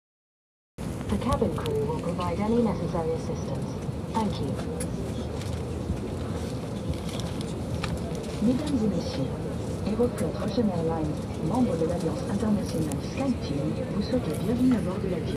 К нам в самолёт сел Джигурда :D
Вот это нежданчик!))